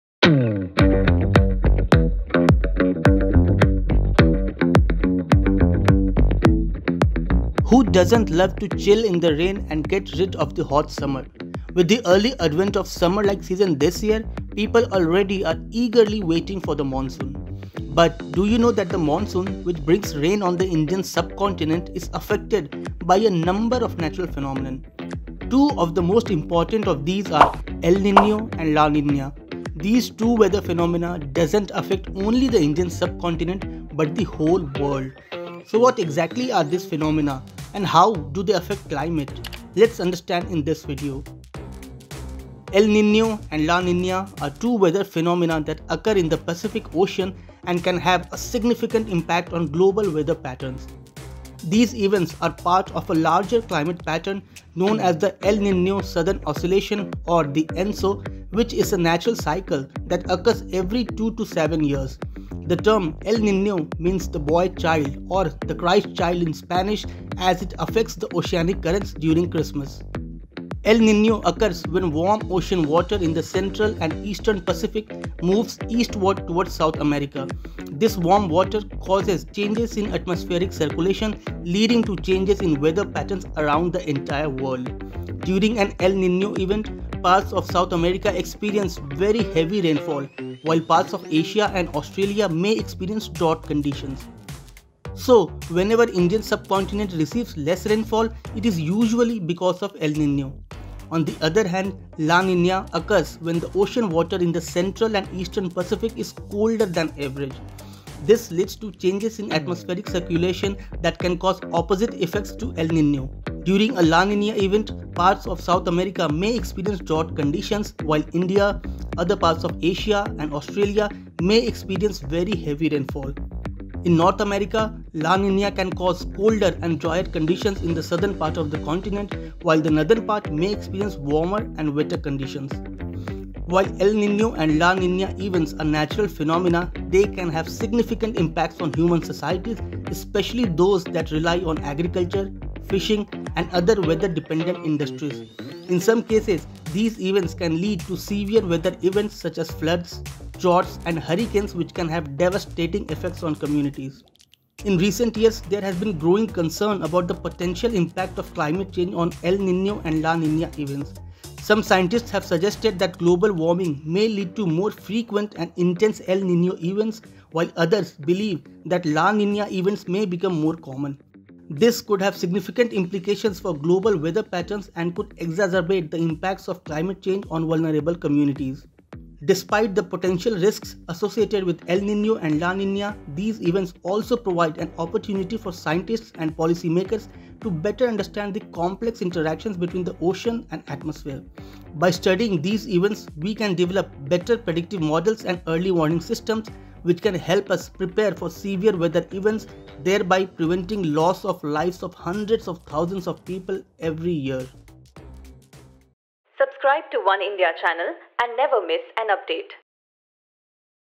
Heatwave Spell: IMD warns of heatwave spells in April | El Nino, La Nina | Oneindia
The seven-phase Lok Sabha polls season will witness an unprecedented rise in the heatwave in most parts of the country, the India Meteorological Department warned here on Monday. While above-normal temperatures are likely almost all over the country, 10-20 days of heatwave is expected between April and June in different parts against normal of 4-8 days. The most heatwave prone areas are Gujarat, central Maharashtra and north Chhattisgarh, Odisha and Andhra Pradesh. “More heatwave days are expected in an El Nino year. The El Nino (an unusual rise of temperature in the Pacific Ocean with impacts all over the world) is still there in April-May even though it is receding and may become neutral by June,” Mrutyunjay Mohapatra, director general of meteorology at the IMD said.

What are El Nino and La Nina and how do they affect the weather conditions?